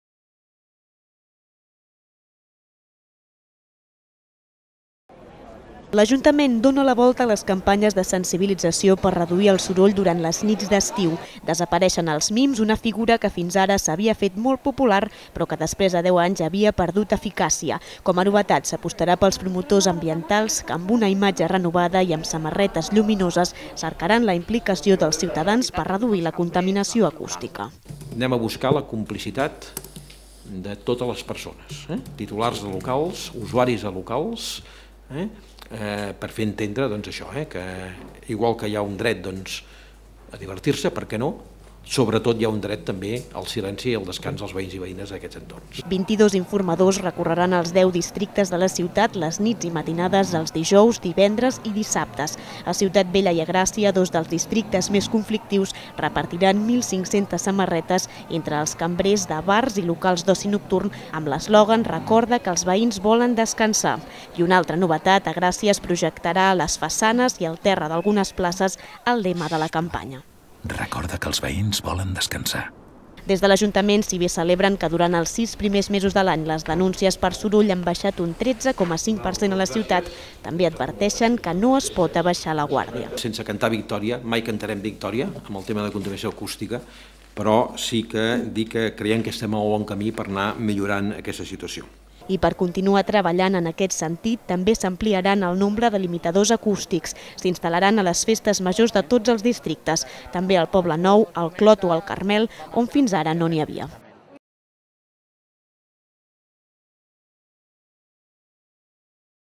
El Pla Silenci busca la complicitat per reduir el soroll al carrer durant l'estiu
Un any més l'Ajuntament posa en marxa la campanya de sensibilització per reduir el soroll durant les nits d'estiu. Enguany 22 informadors recorreran els 10 districtes de la ciutat buscant la complicitat dels ciutadans per contribuir a reduir la contaminació acústica. Durant els sis primers a mesos de l'any les incidències per soroll han baixat un 13,5%.